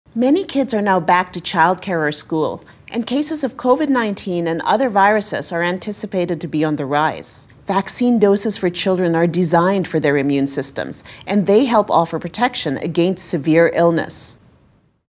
ਡਾ. ਐਨਾ ਵੋਲਾਕ ਬੱਚਿਆਂ ਲਈ ਕੋਵਿਡ-19 ਵੈਕਸੀਨ ਬਾਰੇ ਗੱਲ ਕਰ ਰਹੀ ਹੈ।
ਪੀਡੀਐਟ੍ਰਿਕ ਵੈਕਸੀਨ ਬੱਚਿਆਂ ਦੇ ਇਮਿਊਨ ਸਿਸਟਮ ਲਈ ਤਿਆਰ ਕੀਤੀਆਂ ਗਈਆਂ ਹਨ। ਵੈਕਸੀਨ ਦੀ ਸਖਤੀ ਨਾਲ ਜਾਂਚ ਕੀਤੀ ਗਈ ਹੈ ਅਤੇ ਸੁਰੱਖਿਅਤ ਹਨ।